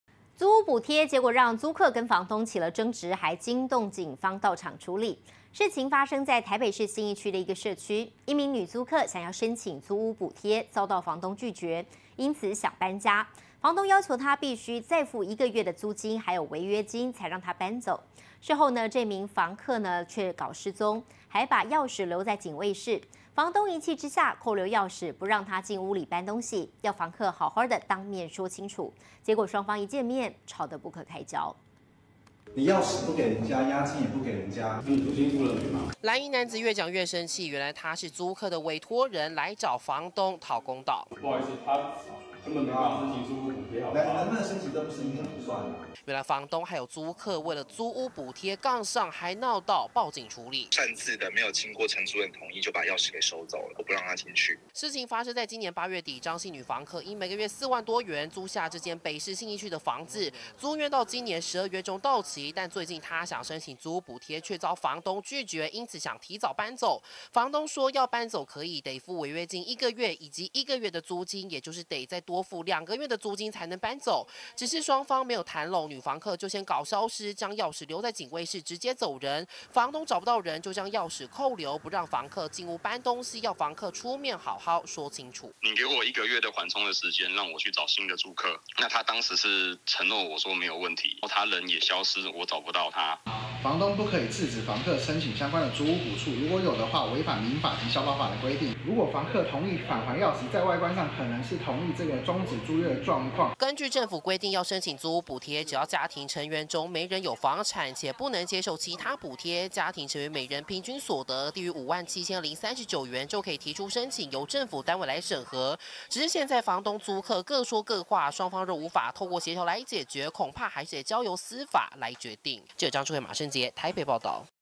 沒付房租不讓搬 房東.租客社區大廳爆發口角
租屋補貼，讓租客、房東起爭執，還驚動警方到場處理。事發台北信義區某社區，一名女租客想申請租屋補貼，卻遭房東拒絕，因此想搬家，房東要求她必須再付一個月的租金，還有違約金，才讓她搬走，但這名房客事後卻失蹤，還將鑰匙留在警衛室。房東一氣下，扣留鑰匙，不讓她進屋搬東西，要房客好好當面說清楚，雙方一見面，吵得不可開交。